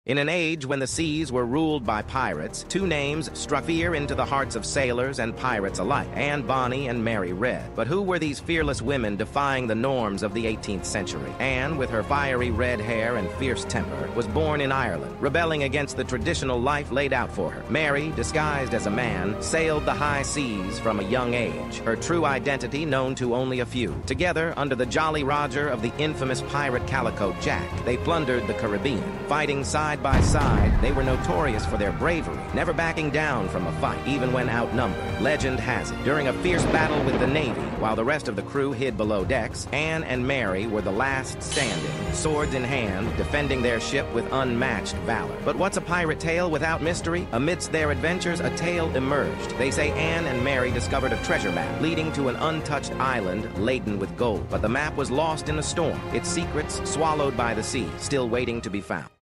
🏴‍☠️Swashbuckling She-Pirates: Anne Bonny & Mary Read's Legendary Treasure Hunt 💰treasurehunting
Dive into the heart-pounding adventures of Anne Bonny and Mary Read, the fearless pirate queens who defied the norms and carved their names into the annals of history. From their bold battles on the high seas to the mysterious treasure hunt that has captivated imaginations for centuries, this video brings their legendary story to life. Join us on a journey through time and uncover the truth behind the myths.